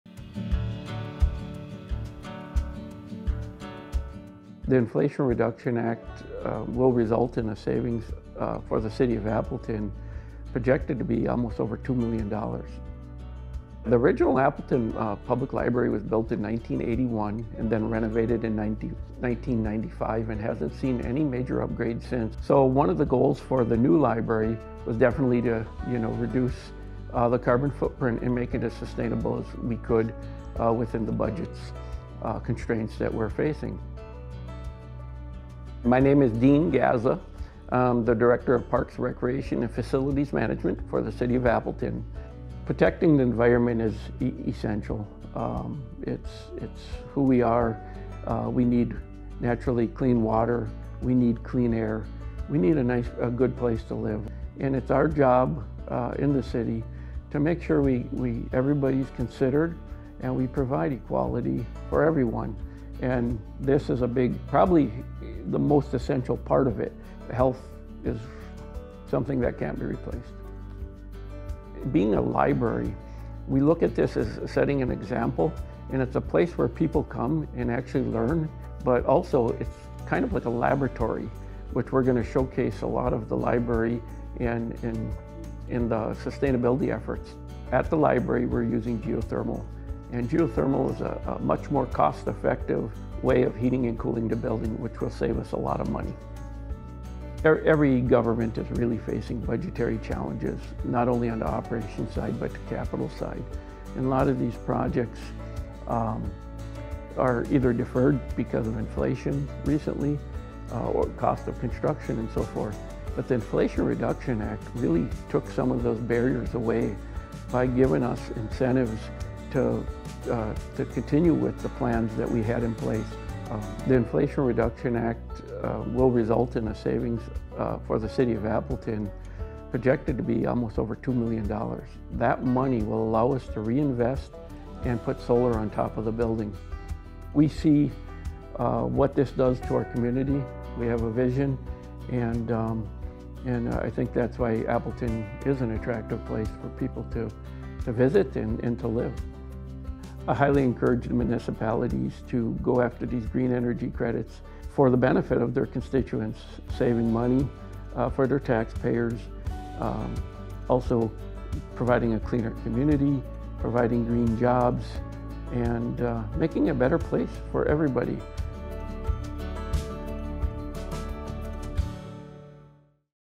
Appleton's new sustainable library is benefiting city residents | WCV Stories
Thanks to Pres. Biden's Affordable Clean Energy Plan, the City of Appleton was able to save almost two million dollars in the construction of their new library downtown. With those savings, the city will reinvest that money to put solar panels on the roof of the new library in addition to the already planned energy-efficient geothermal heating and cooling.

Dean Gazza, the director of Parks and Recreation and Facilities Management at the City of Appleton, explains that the library will be a learning center for the community and showcase the renewable energy the building is using. Not only will this project save the city money in the long run, but this building puts the city on track towards its clean energy goals and benefits the health of its residents.

Learn more about how you can take advantage of these benefits

Wisconsin Conservation Voters engages voters to protect Wisconsin's environment.